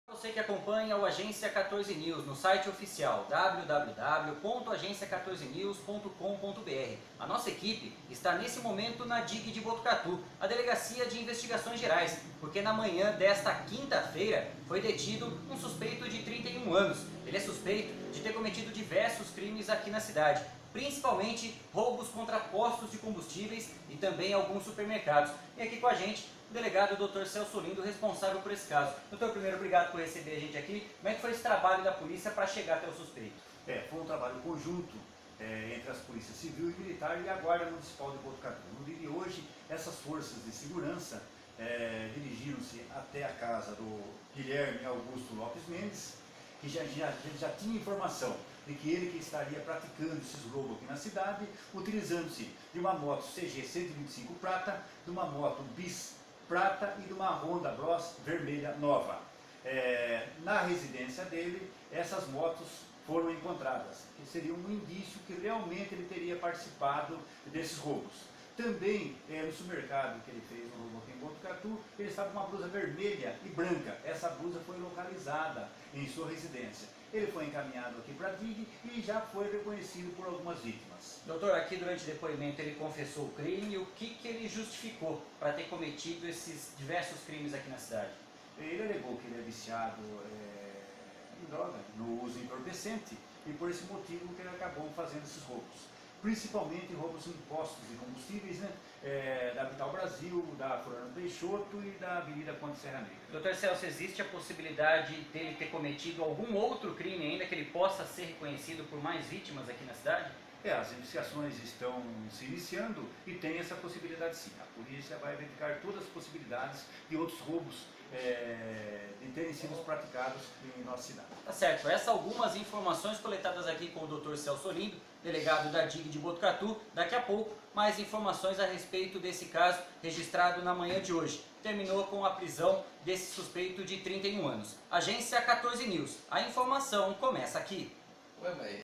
Suspeito é preso acusado de roubar assaltos a postos de combustíveis e supermercado
O Jornalista Cleber Novelli entrevista o delegado Celso Olindo.